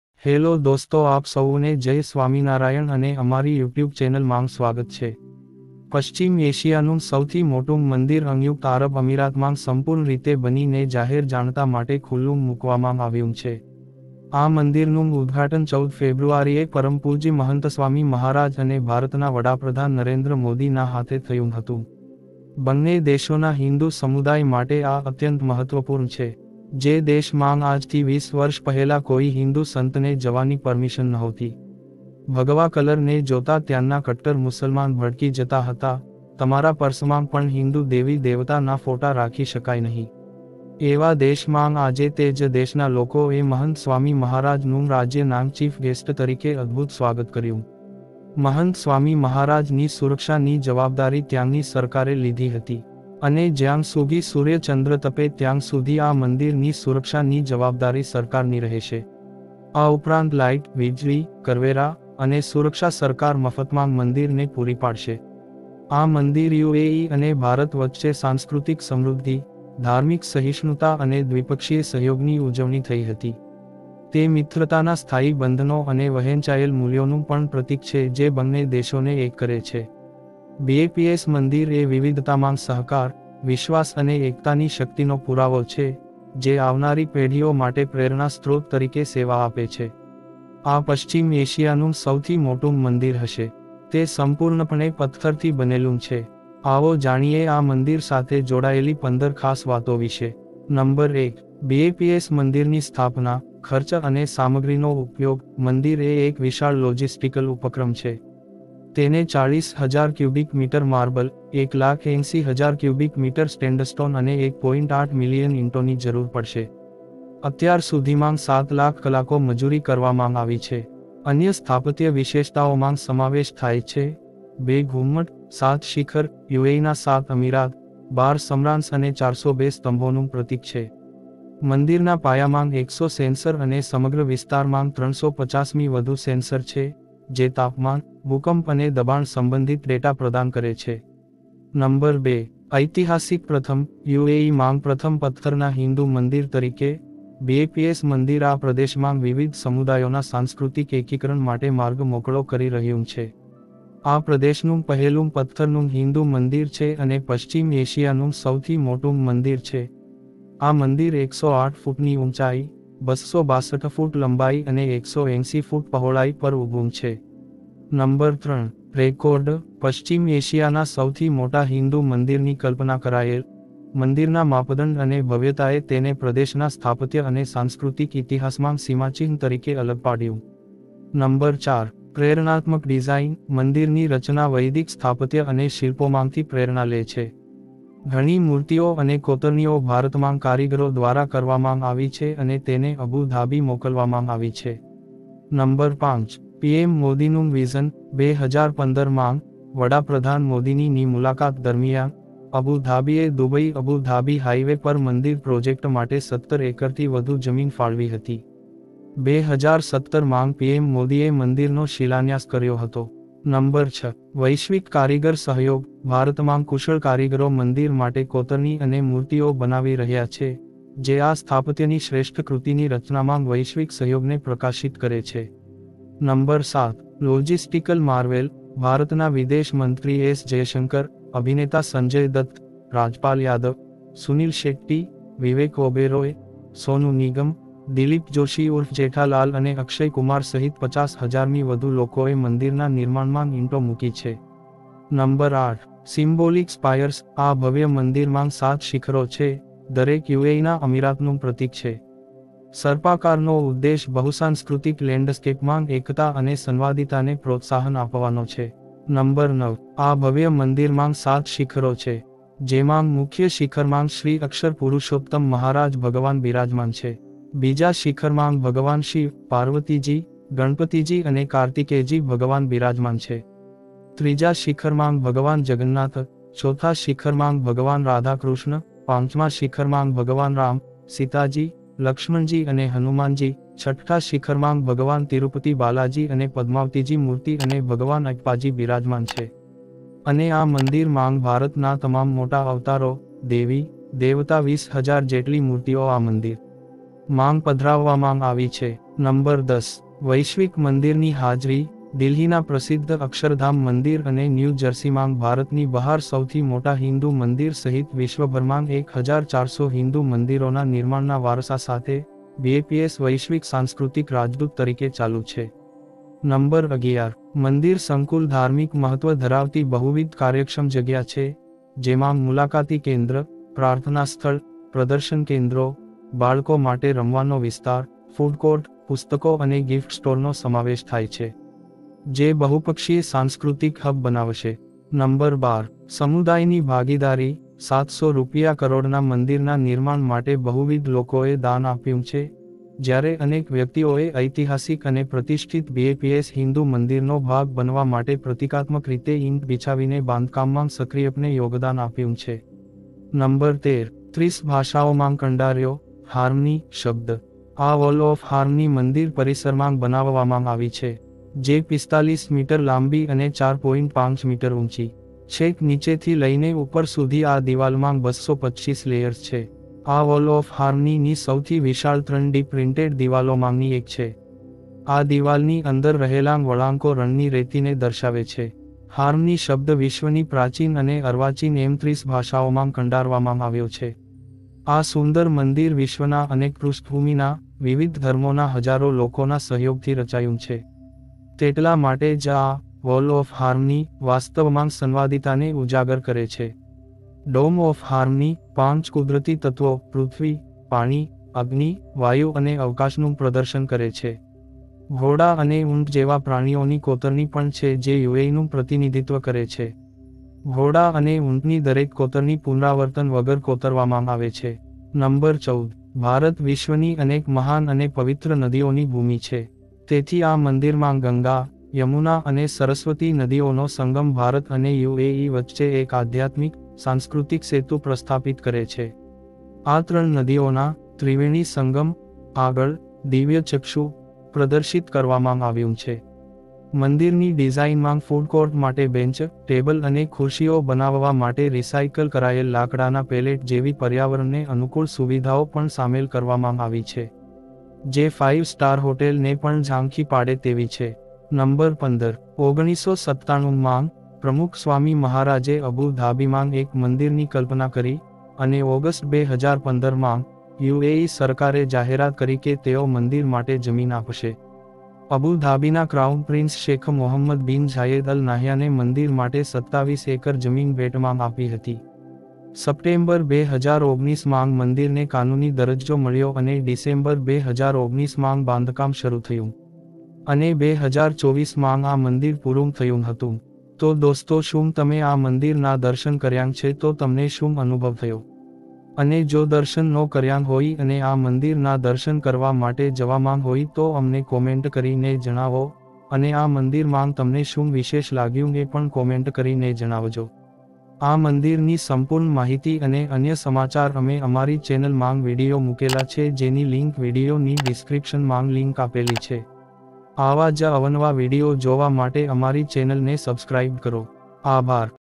સનાતન ધર્મના અબુધાબી હિંદુ મંદિરની 10 અજાણી વાતો! Top 15 Interesting Facts About UAE BAPS Mandir
15 Must-Know Facts About the Grand BAPS Hindu Temple in Abu Dhabi | Top 15 Interesting Insights

🕊️ Unlock the secrets of the magnificent BAPS Hindu Temple in Abu Dhabi! 🕌 Join us as we explore the top 15 interesting facts about this historic monument, inaugurated on February 14, 2024, by His Holiness Mahantaswami Maharaj and Prime Minister Narendra Modi. From record-breaking architecture to global collaborations, this temple is a symbol of cultural integration and friendship between the UAE and India. Dive into the details that make this temple a true marvel and a testament to unity in diversity! 🌐🇦🇪🇮🇳

Abu Dhabi Mandir Video